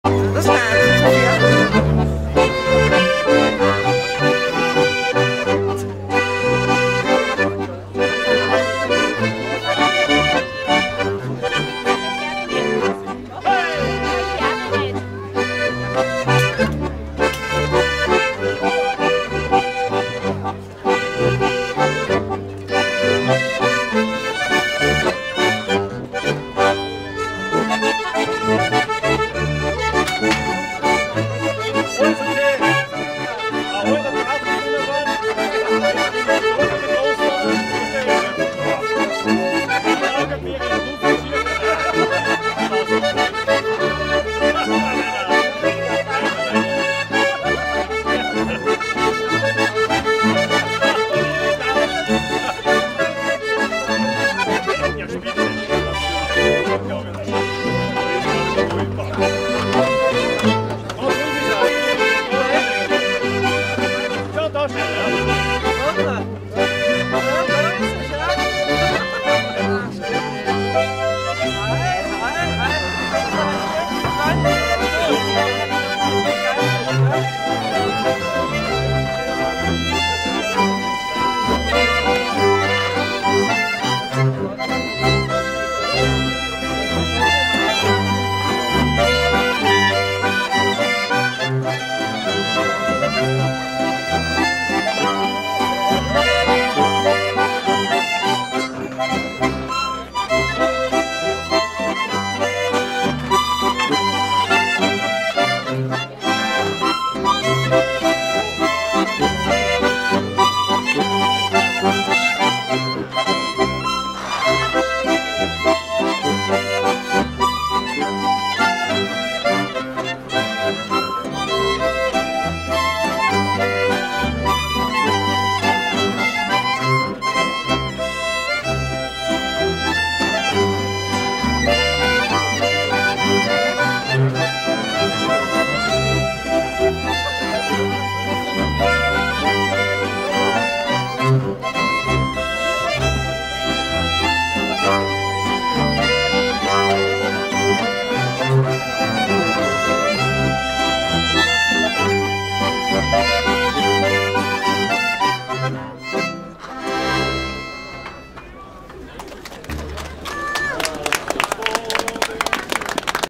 Brandl Geigenmusi 8-1 © OlmHERZ
Brandl Geigenmusi bei Waldweihnacht in Bruck a.d. Mur, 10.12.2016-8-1 © OlmHERZ